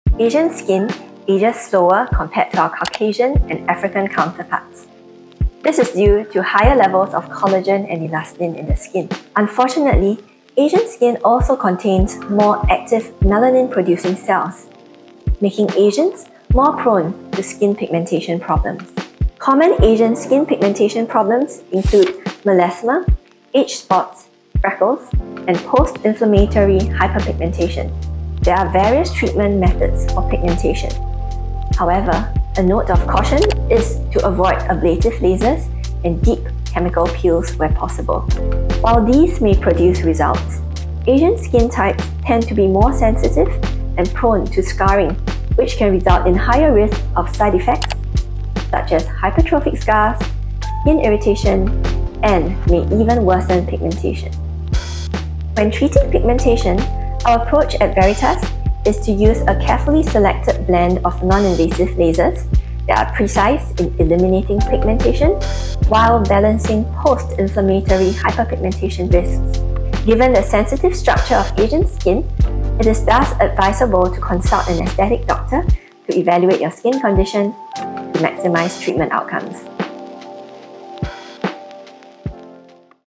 Why is Asian Skin More Prone to Pigmentation?
Dr Lena Fan (Veritas Medical Aesthetics) explains what makes Asian skin more prone to pigmentation and what to caution when treating delicate Asian skin.